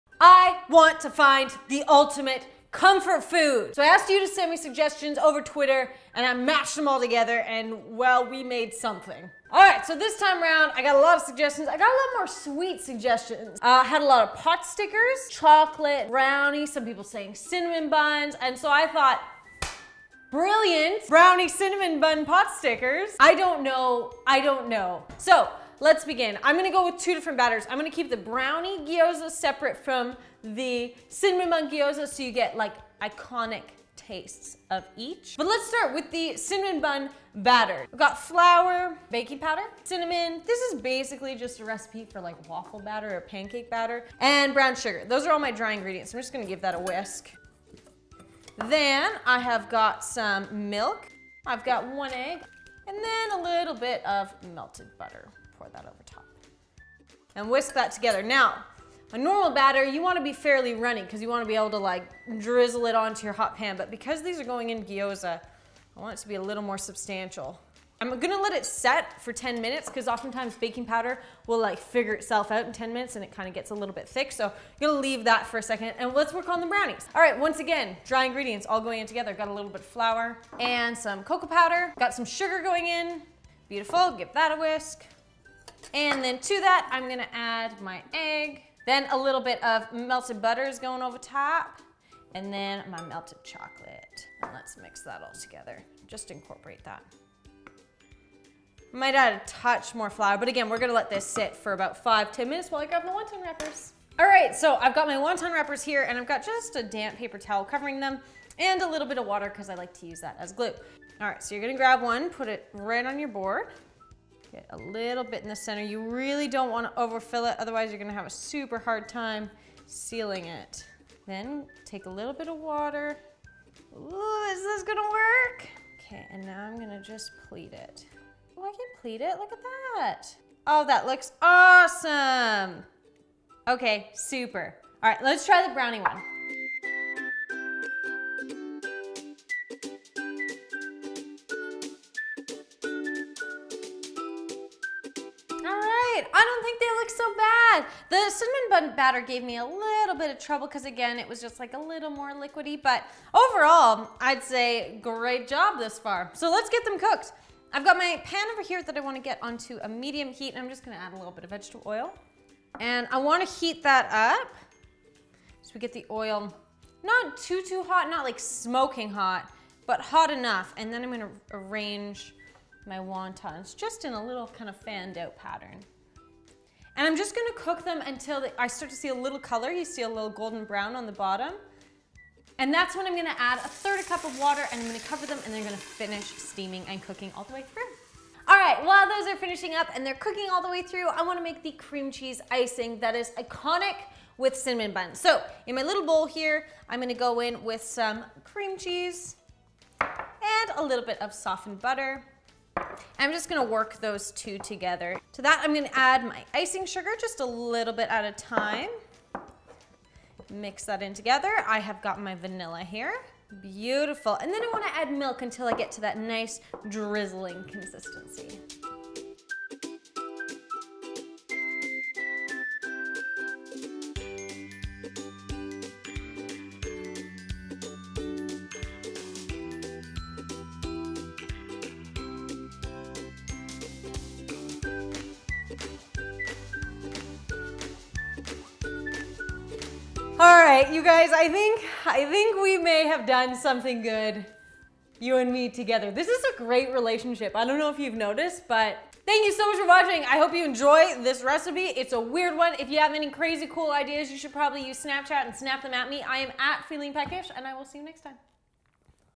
ASK THE AUDIENCE: Finding the Ultimate Comfort Food | 5 Second Rule with Julie
In her quest for the ultimate comfort food, Julie asked viewers for their favorites. This mash-up is chocolate and cinnamon magic. I mean, who wouldn't want to try a Brownie Cinnamon Bun Potsticker?!

INGREDIENTS:
1 pkg. Wonton Wrappers
2 tbsp vegetable oil
1/3 cup water
Hot Fudge
Cinnamon Bun Filling:
1 ½ cups flour
2 teaspoons baking powder
1 cup milk
1 large egg
4 tablespoons butter, melted
1/4 cup packed light brown sugar
1/2 tablespoon ground cinnamon
Brownie filling:
¼ cup of flour
1/3 cup of Sugar
1/3 cup of cocoa Powder
¼  cup of Butter
35g Dark Chocolate, melted
1 egg
Cream cheese glaze:
4 tablespoons (1/2 stick) unsalted butter
2-ounces cream cheese, at room temperature
3/4 cup powdered sugar
1/2 teaspoon vanilla extract

STEPS:
1.) Place all the ingredients for the cinnamon bun filling in a bowl and whisk together until smooth. Set aside for 10 minutes. Repeat for the brownie batter filling.
2.) To fill wonton wrappers place 1 tbsp amount of either the cinnamon filling or brownie filling in the center and then dip a finger in some cold water and run it along the outer edge as glue. Be careful not to over fill. Bring the outside edges together to seal the filling within in whichever way you’d prefer but ensuring no filling can seep out. Place a small amount of oil in a saucepan and heat over medium. 3.) Place wontons in pan in a fanned out circle and cook until they’ve browned slightly on the bottom. Pour in 1/3 cup water and cover with a lid for 5-7 minutes or until all water is evaporated.
4.) For the cream cheese glaze combine all ingredients and blend using an electric mixer until smooth. Drizzle over the cinnamon bun potstickers and finish with a dusting of cinnamon. To finish the brownie potstickers drizzle with some hot fudge.